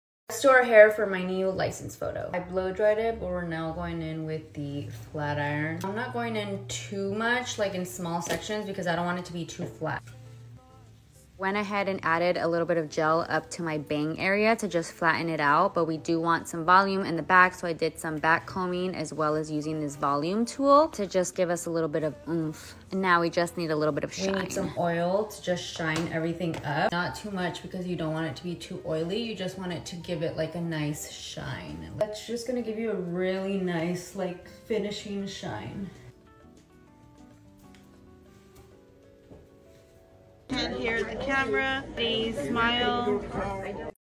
Let’s do our hair for Drivers License Photo | YesHipolito
Sleek look for my new drivers license! Don’t sleep on hair oil after styling!